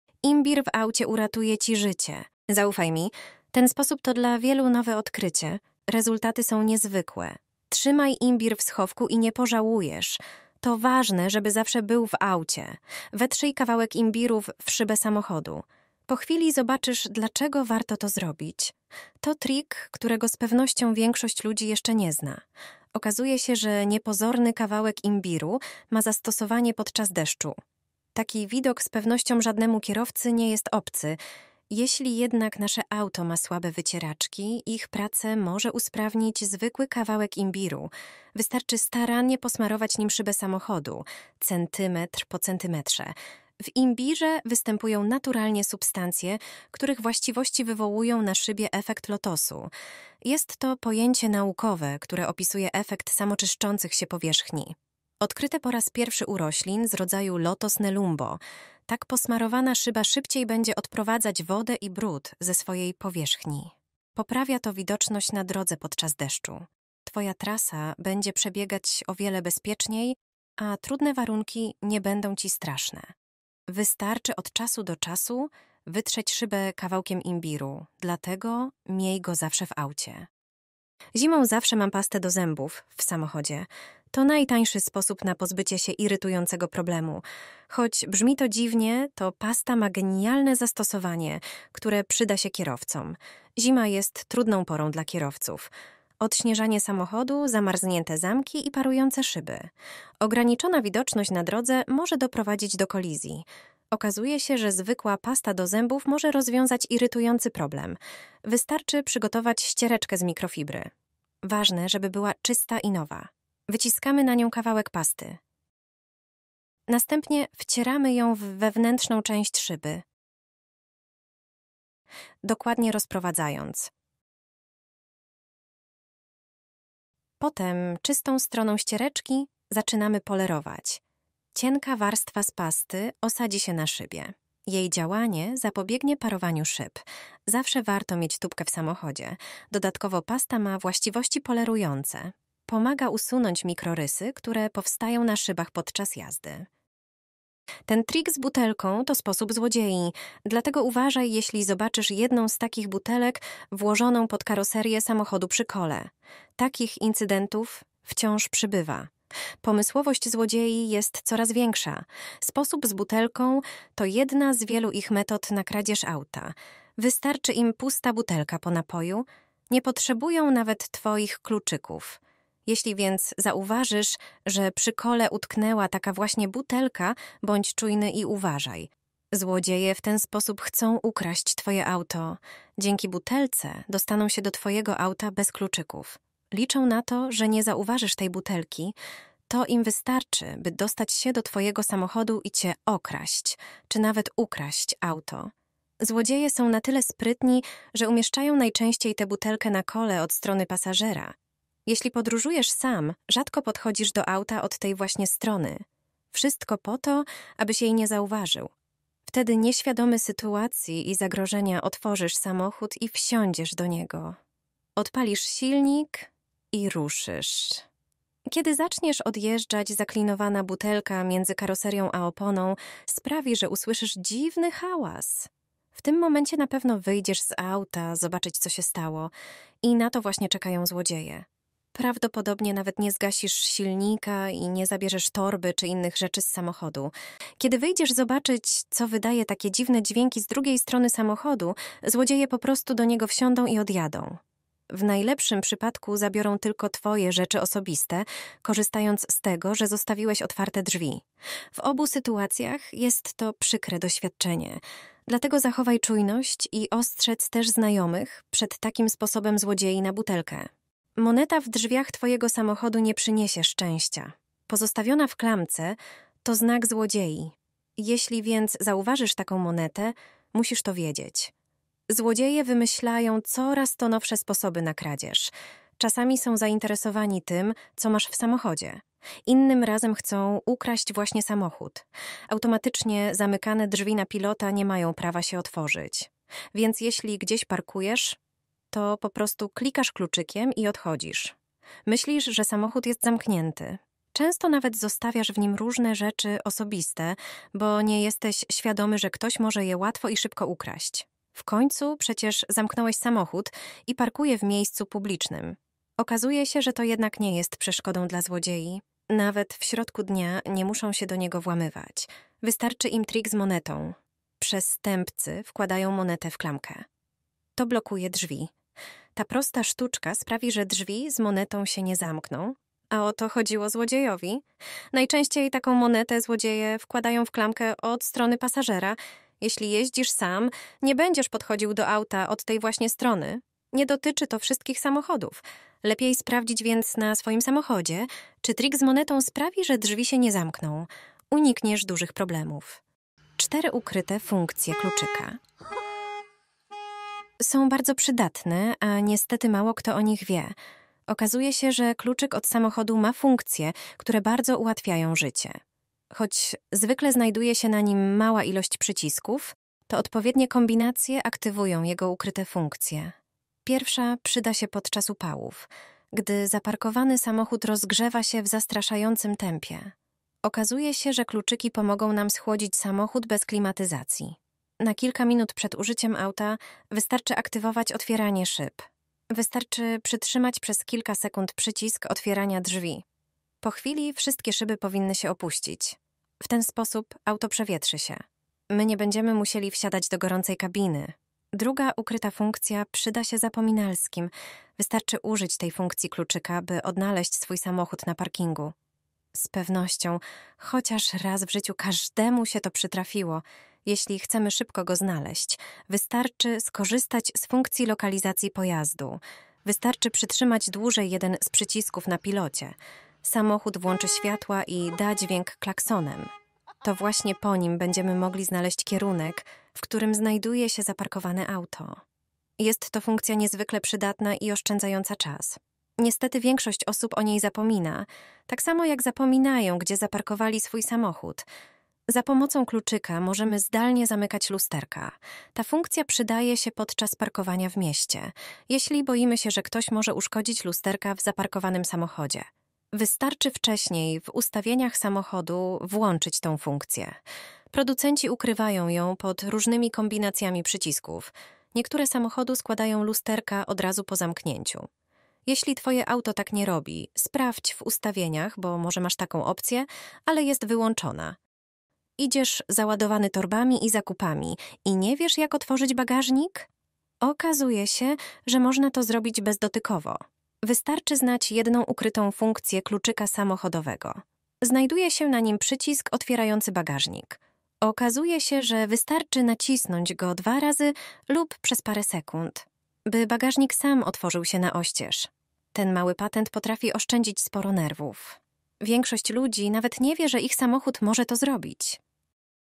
Imbir w aucie uratuje ci życie. Wystarczy trzymać go w schowku
Zawsze trzymam imbir w schowku. Ten sposób to dla wielu nowe odkrycie. Rezultaty są niezwykłe. Trzymaj imbir w schowku i nie pożałujesz! To ważne, żeby zawsze był w aucie!